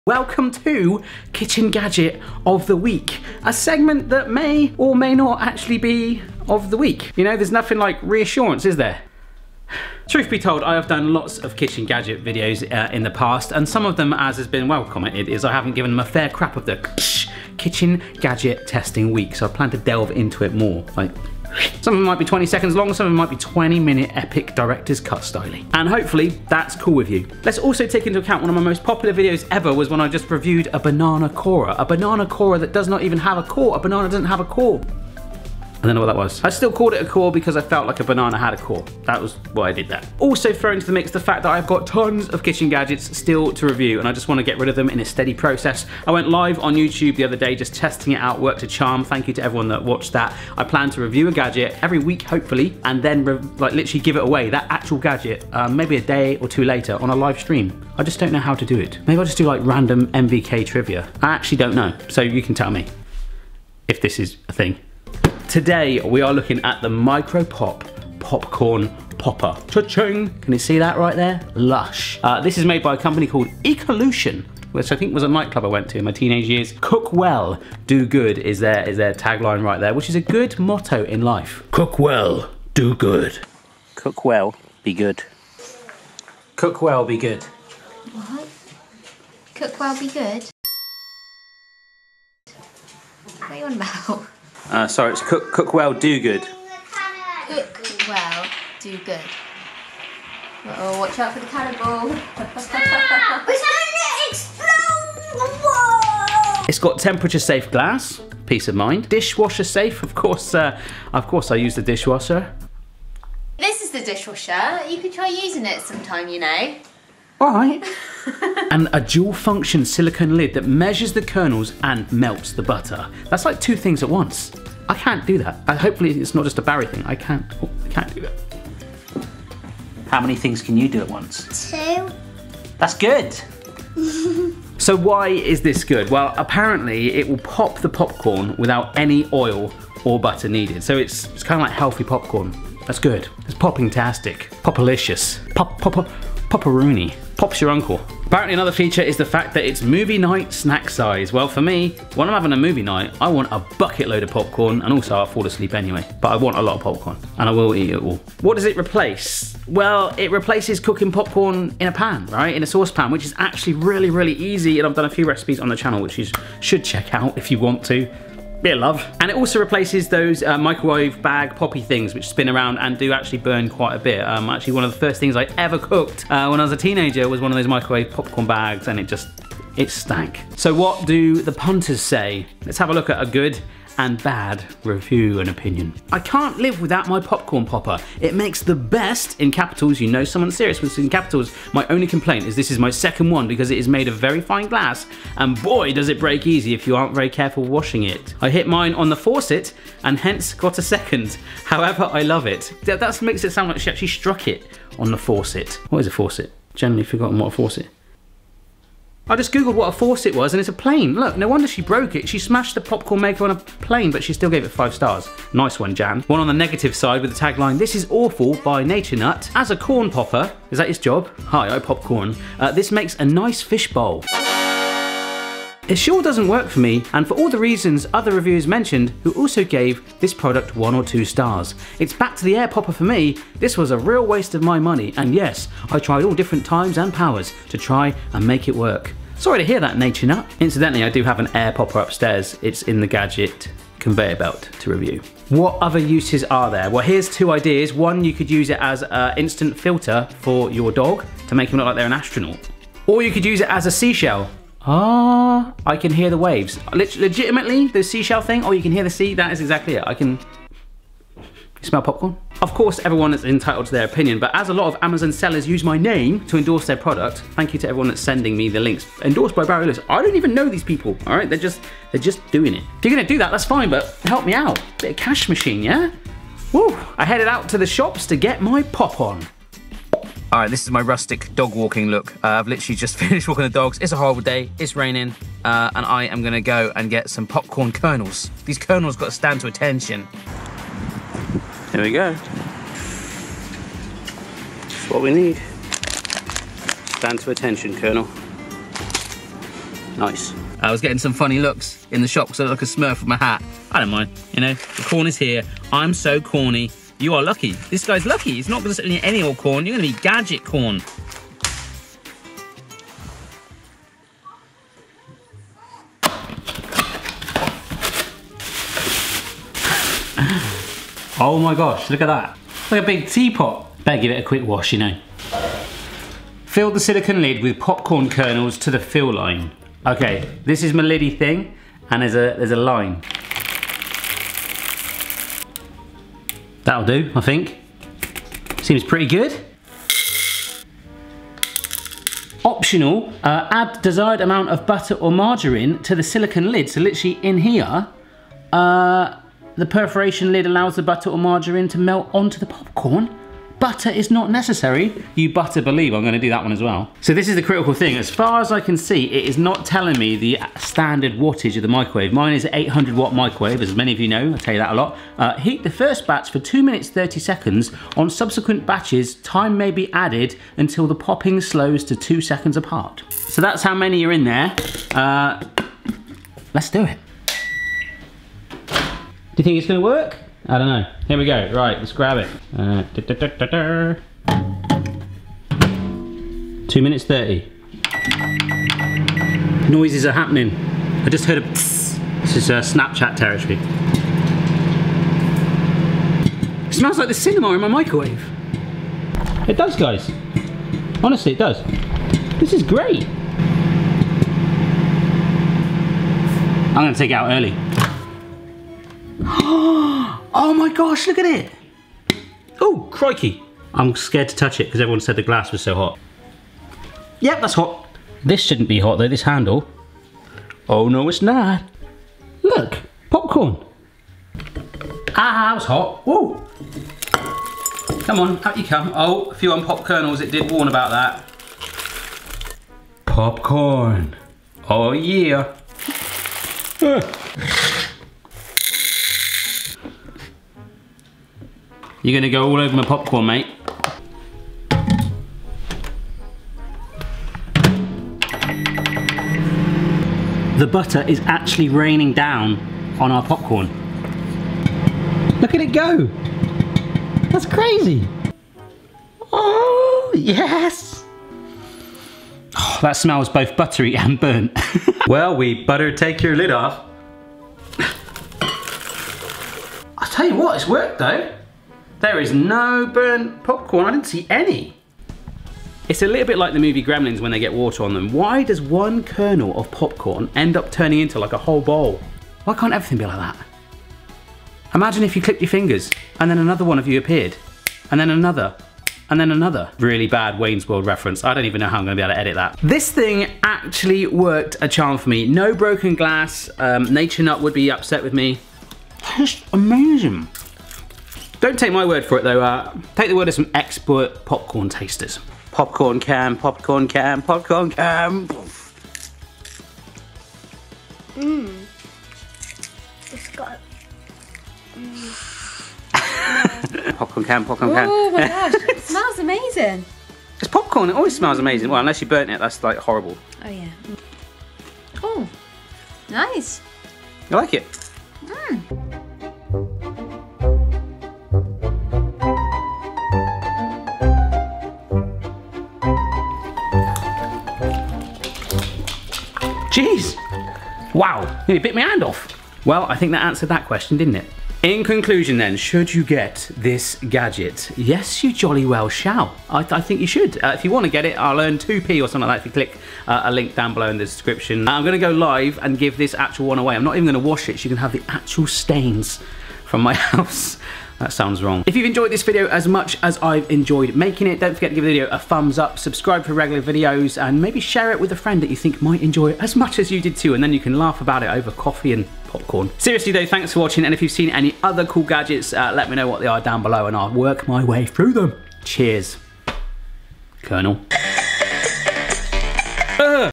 Is this the best Popcorn Maker?
Kitchen Gadget Testing the Micro popcorn popper. Unboxing & putting to the test this microwave popcorn gadget that pops your corn in 2 minutes in a little bowl! What's your fave kitchen gadget for me to review?

► If you would like to help me translate any of my videos to a different language